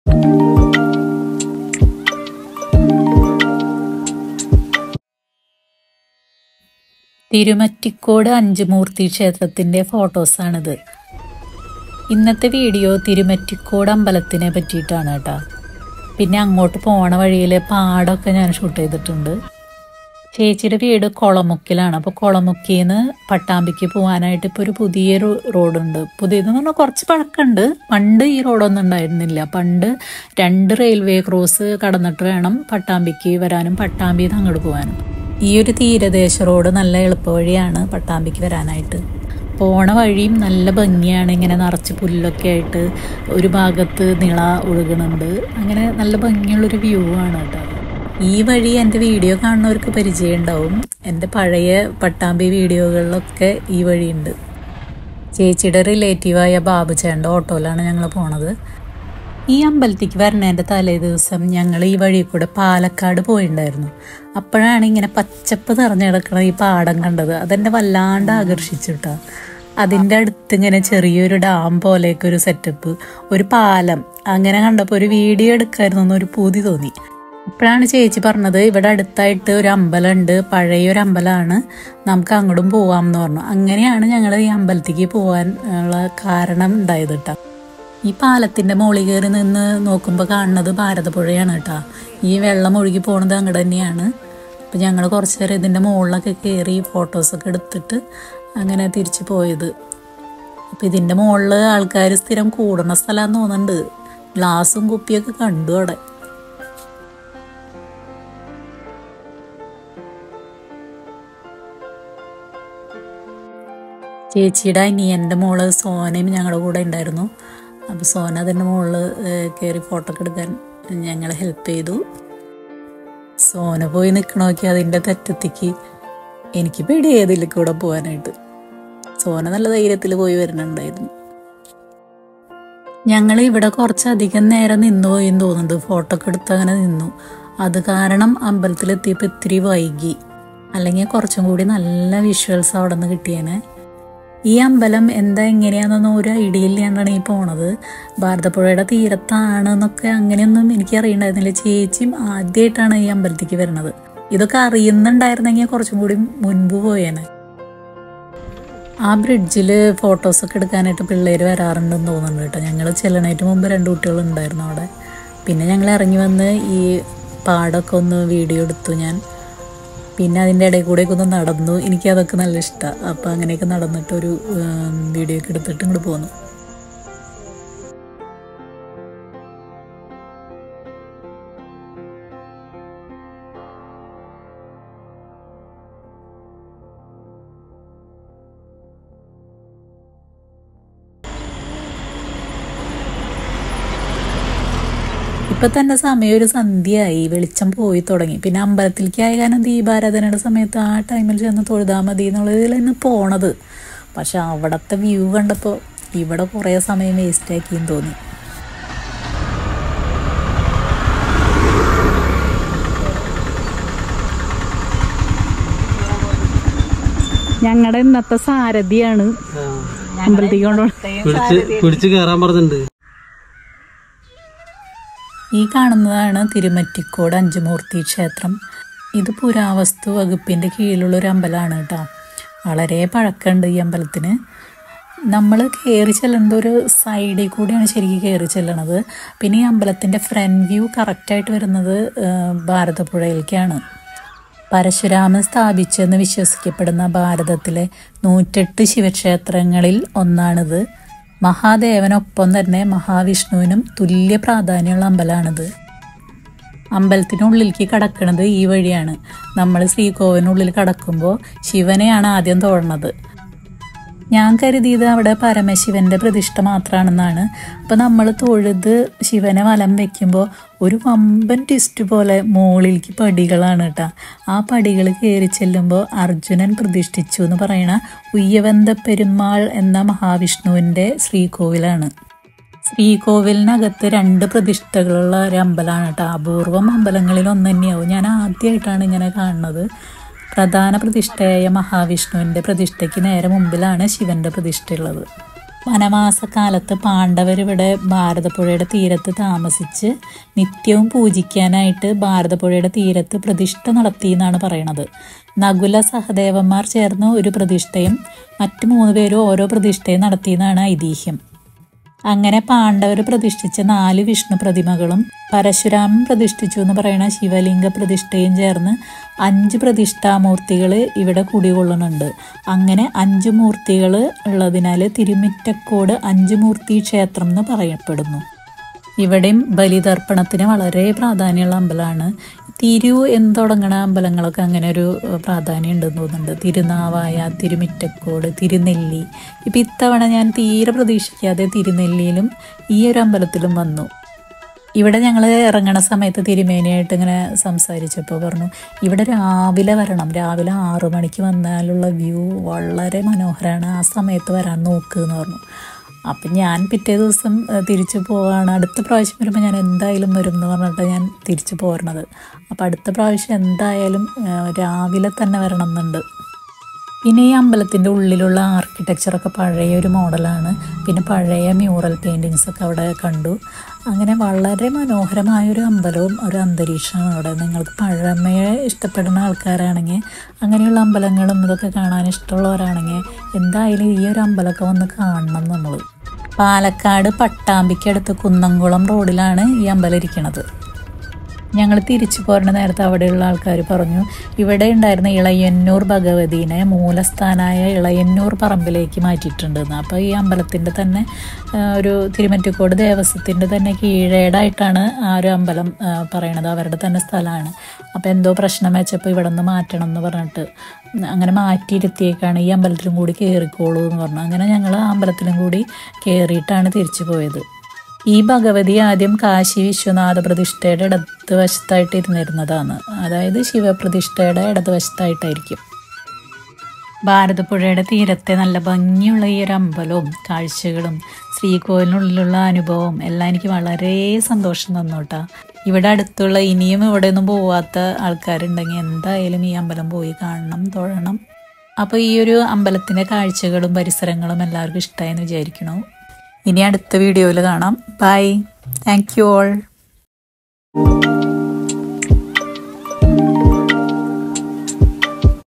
നിളാതീരത്തെ തിരുമ്മിറ്റക്കോട് അഞ്ചുമൂർത്തിക്ഷേത്രം/Thirumittakodu Anjumoorthi temple
Thirumittakode Anchumoorthi Temple (also called Thiruvithuvakoodu) is a Hindu temple equally associated with the deities Vishnu and Shiva. Temple is situated on the banks of the Bharathappuzha at Thirumittacode of Palakkad District in Kerala state in India. The five statues - one for Shiva and four Vishnu - are known here as Thirumattikodu five (Anchu) Moorthy Temple.

The old architecture has been well preserved, with the nature providing a serene atmosphere and bharatapuzha flowing in the background ❤️❤️